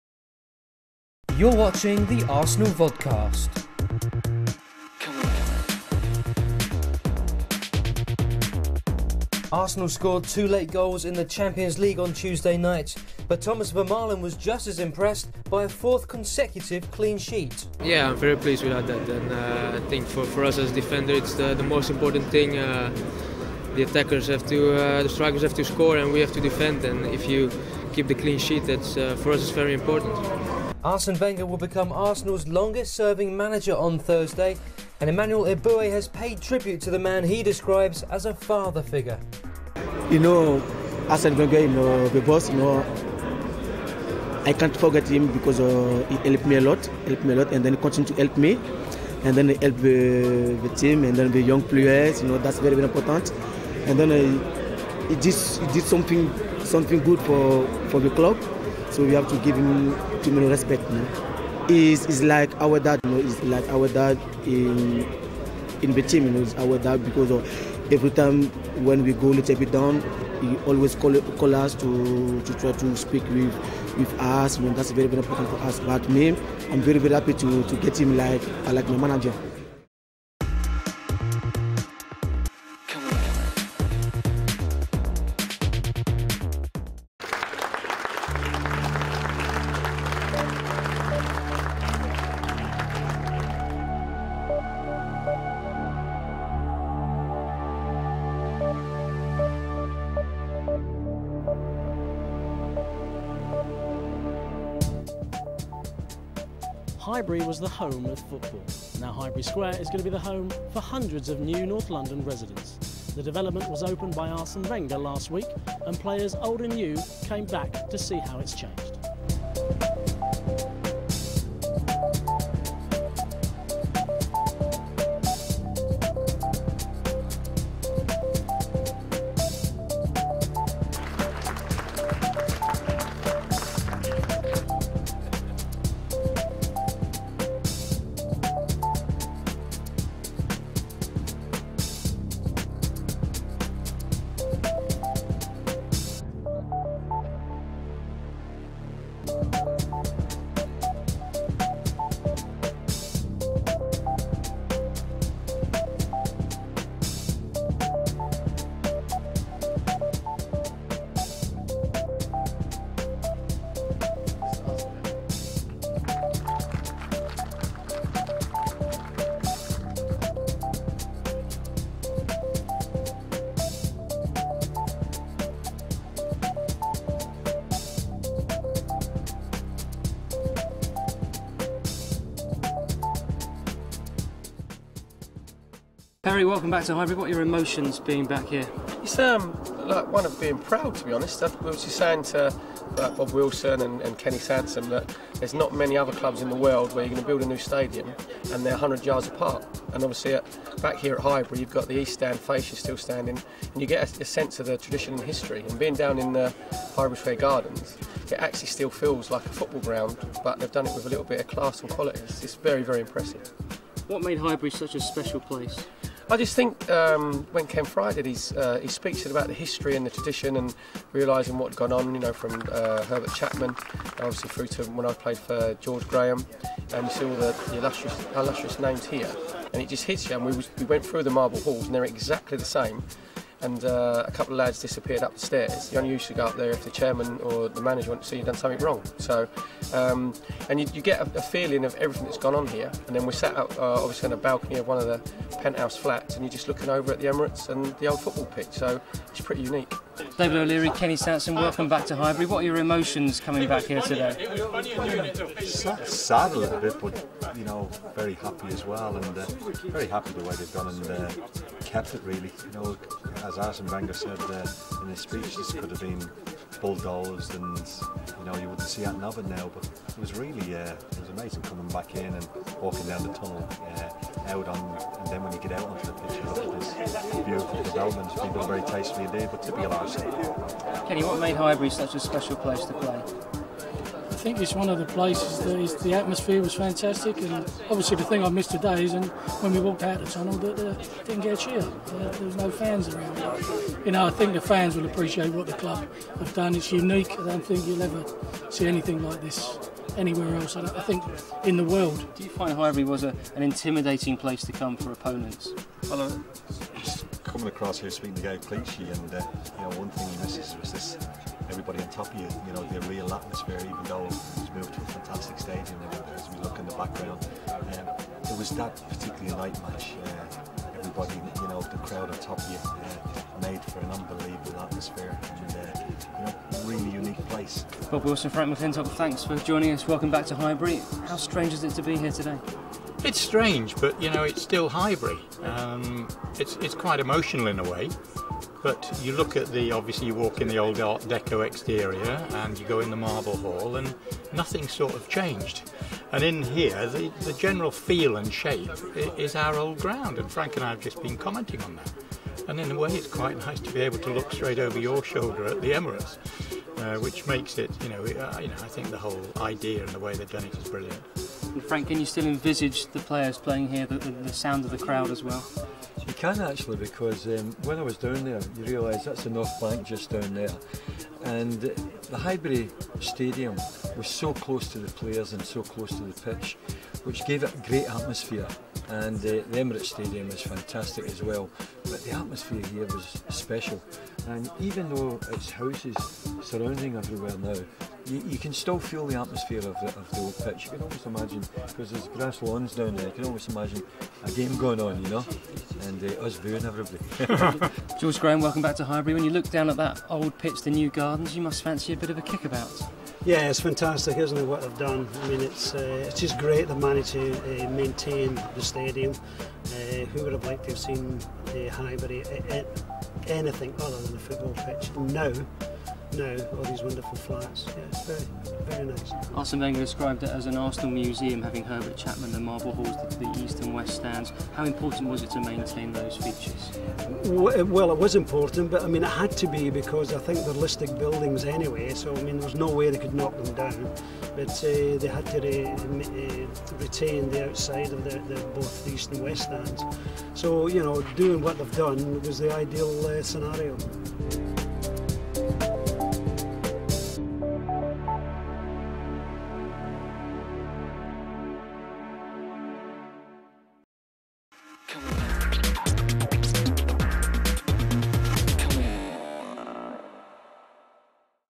o2 arsenal vodcast 30092009
# Thomas Vermaelen on a fourth consecutive clean sheet
# Emmanuel Eboue on Arsène Wenger's new managerial record
# Feature: Arsenal legends at the Highbury Square launch